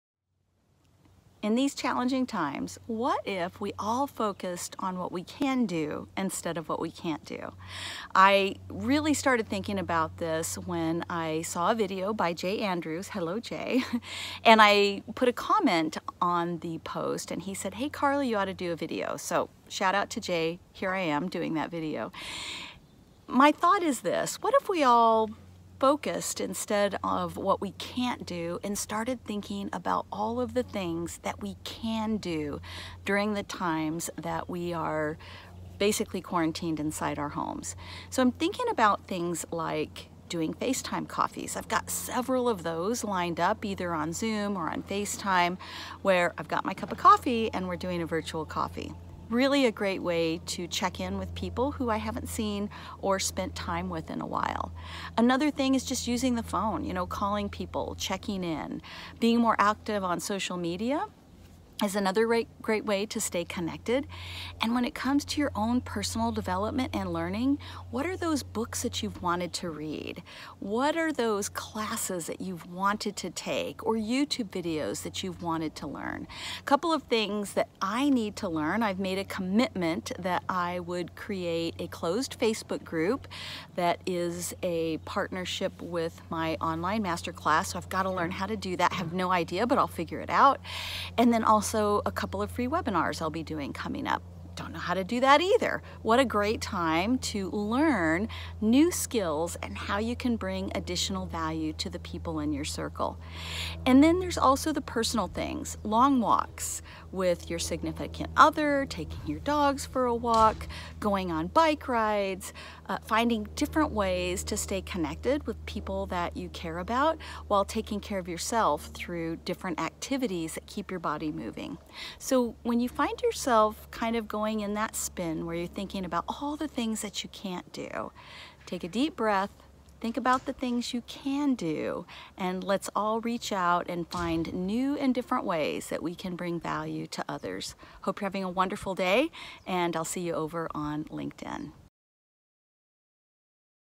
Focusing on YOU... and what you CAN do...
These are certainly challenging times with the COVID19 changing our daily routine! It's easy to get caught up in thinking that doesn't serve us.

What if we all switched our focus to what we CAN do... and focused on how we can grow and learn over the few weeks?

Let's make this a time of discovery and learning. It's up to us! 💖💫